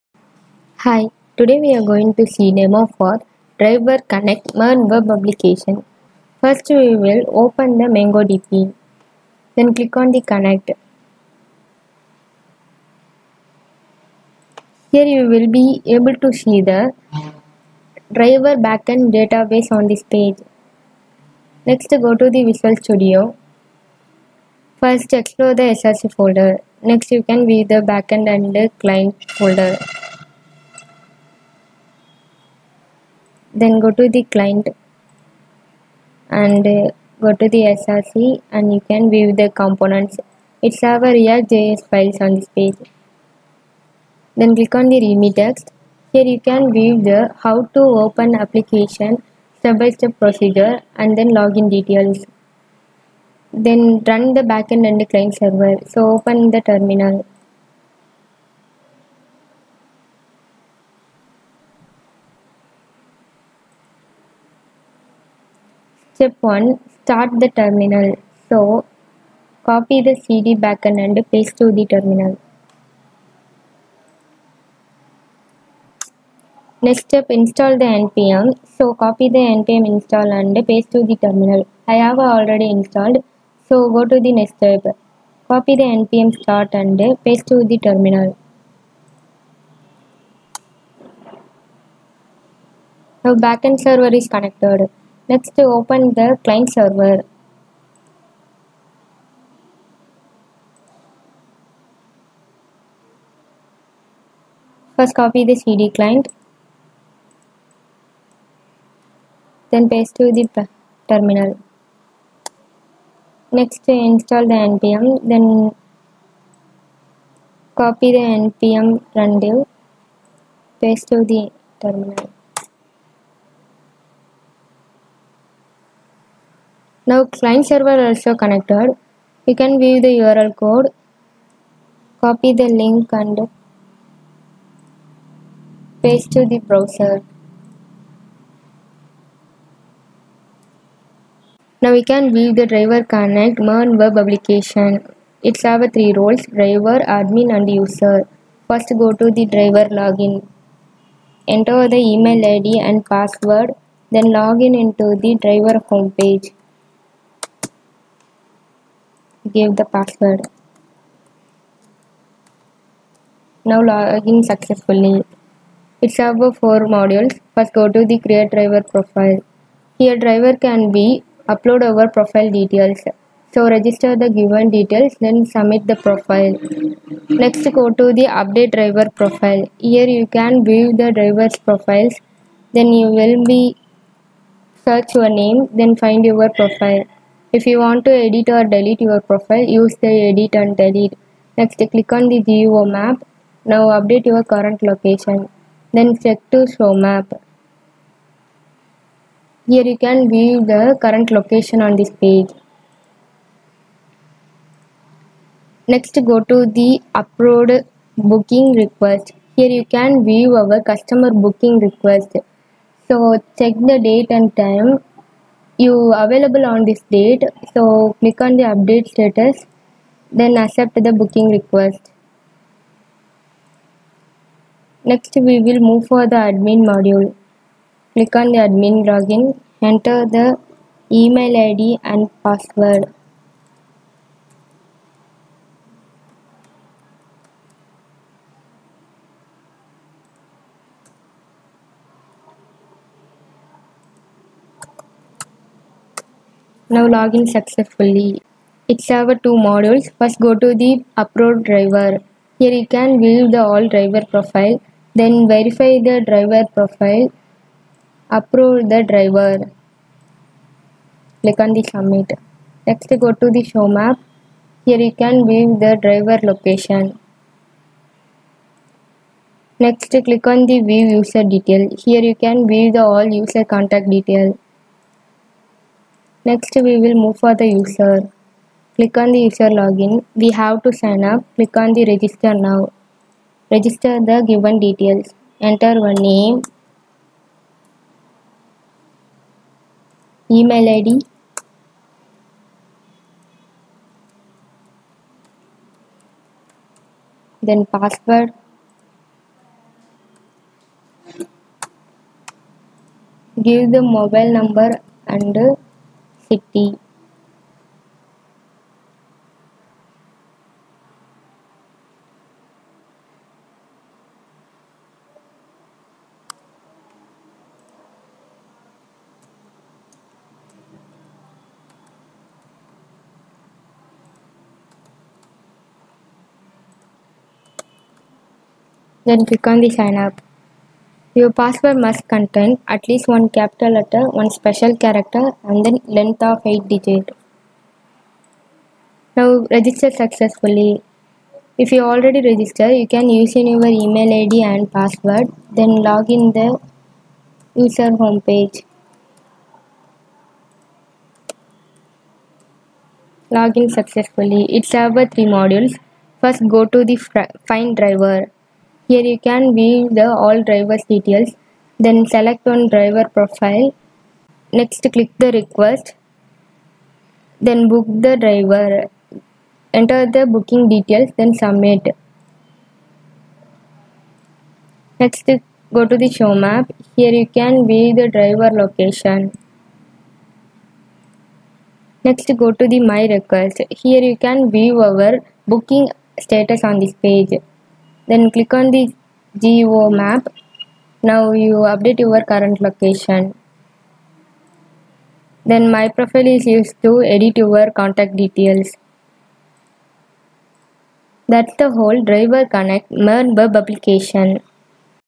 Online Driver Hiring Management System MERN Stack React js Web Application
Online Driver Hiring Management System MERN Stack Web Application
Welcome to our tutorial on creating an "Online Driver Hiring Management System MERN Stack Web Application." In this video, we will guide you through the development of a robust and scalable driver hiring management system using the MERN stack (MongoDB, Express.js, React.js, Node.js). This application will help manage driver profiles, job postings, and bookings efficiently.